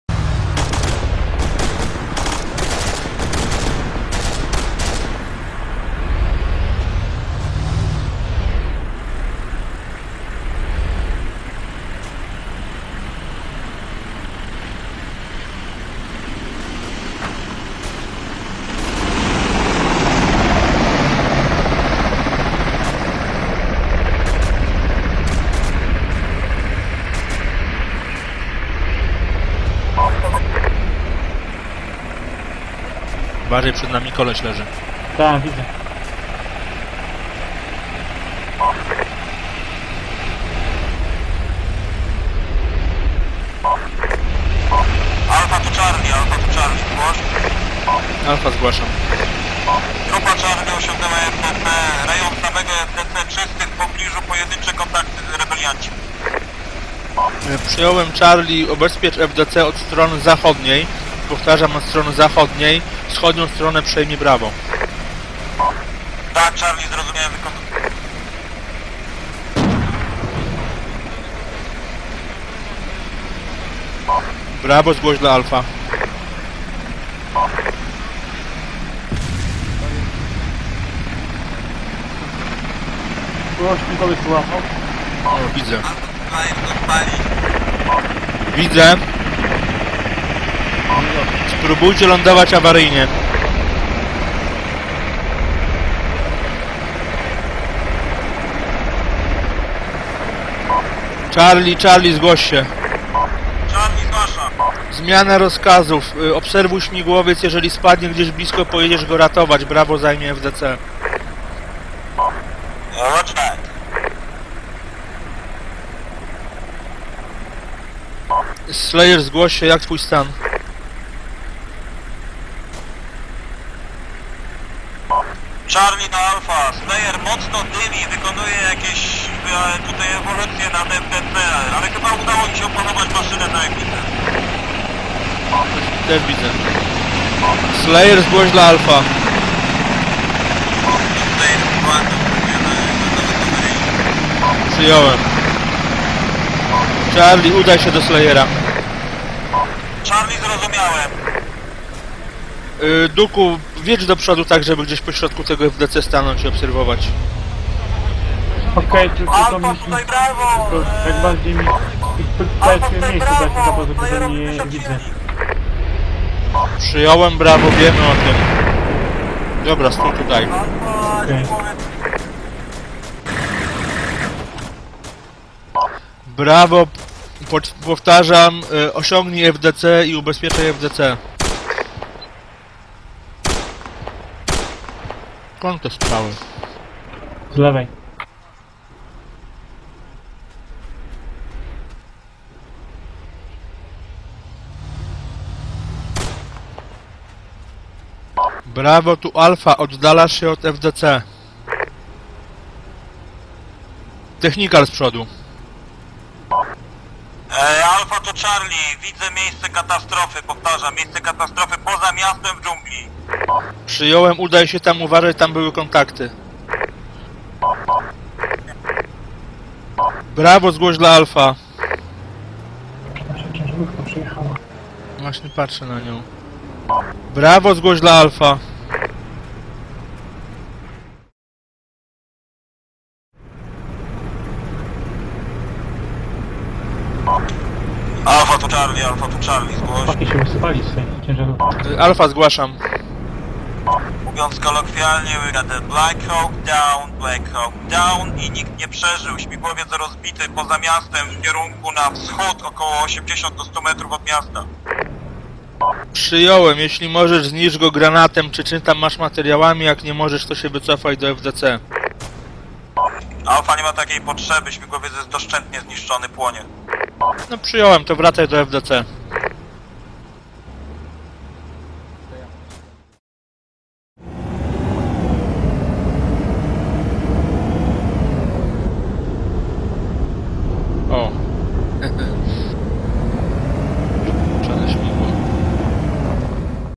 Food Distribution Center, 06.08.11 by ACPL
Scroll down for english info.

===Polski
Misja: Food Distribution Center
Autor misji: Jager
Autor nagrania: Foka

Misja rozegrana 06.08.11
Somalia. Ochrona centrum wydawania żywności, prowadzonego przez ONZ. W czasie zbliżania się do FD wspierający nas Blackhawk został zestrzelony.

===English
Mission: Food Distribution Center
Mission by: Jager
Video by: Foka

Mission played 06.08.11
Somalia. Orders were to go to the Food Distribution Center and protect UN forces distributing food. Supporting Blackhawk was shot down near FDC.

, part of the site.